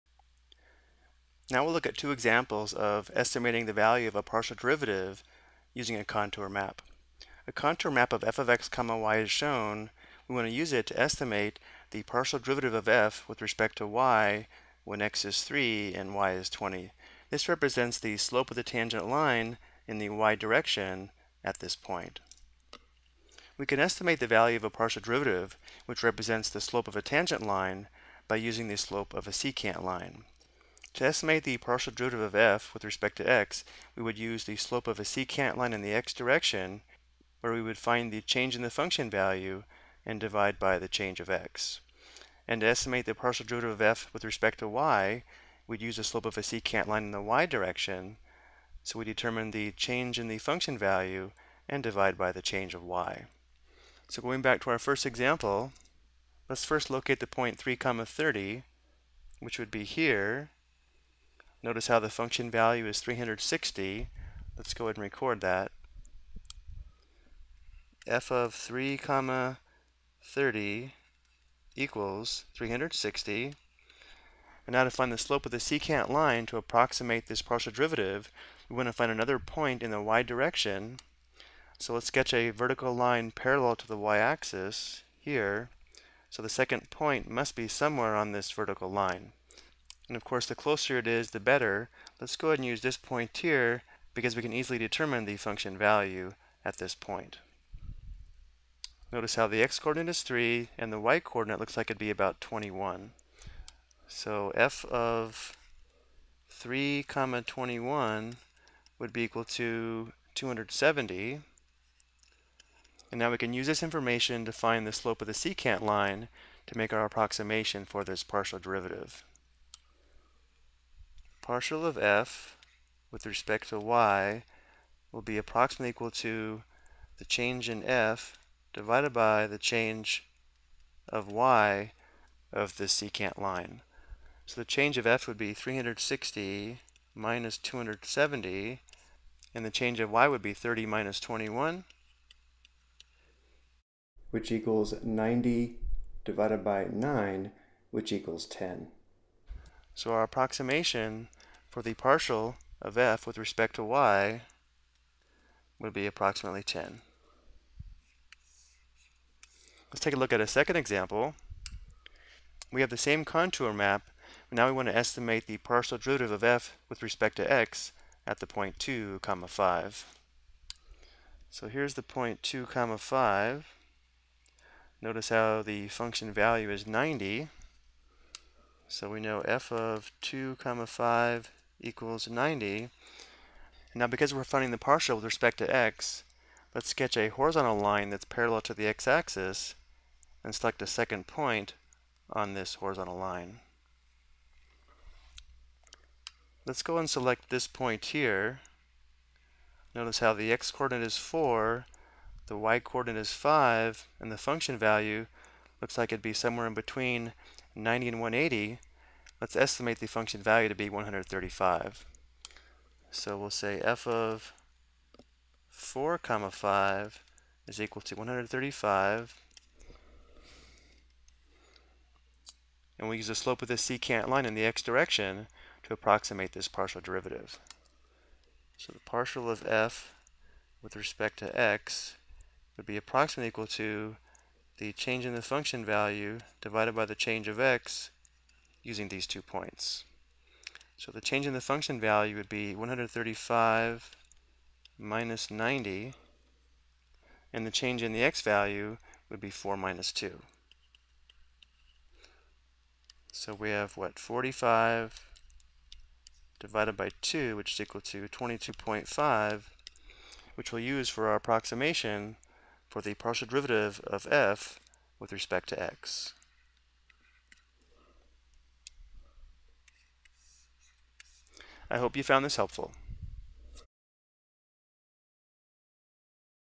Ex: Estimate the Value of a Partial Derivative Using a Contour Map
This video explains how to use the slope of a secant line to determine the value of a partial derivative using a contour map.